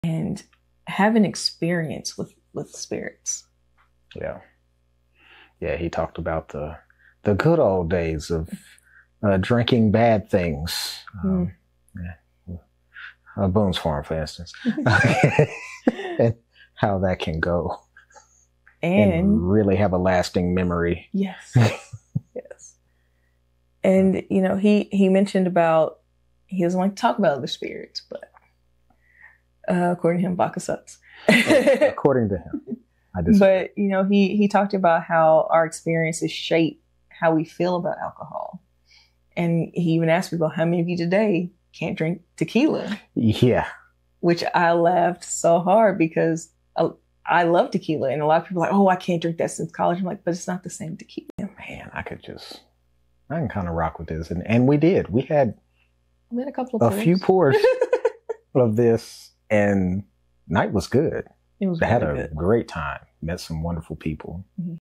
How Alcohol Influences Us
Join us as we delve into spirited conversations about personal experiences with alcohol! From unforgettable college memories to the favorite spirits of our social gatherings, this video captures the joy of sharing stories over drinks. @hochatowndistillery @fredminnick
+21 A true connoisseur knows their limit, drink responsibly!